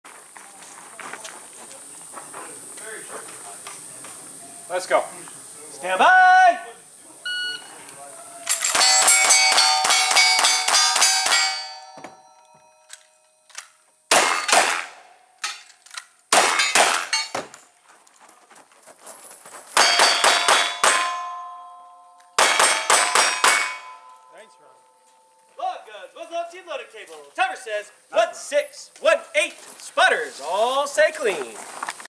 Stage 2 Fireball LCP 8 16 20
Posse 2 - Lake County Pistoleros - August Monthly 2020
Top 5:
1) Santa Fe River Stan
2) Delta Glen
3) The Outlaw Travis James
4) Fireball
5) Crane Creek Kid
Clean Shooters: Poinciana Pete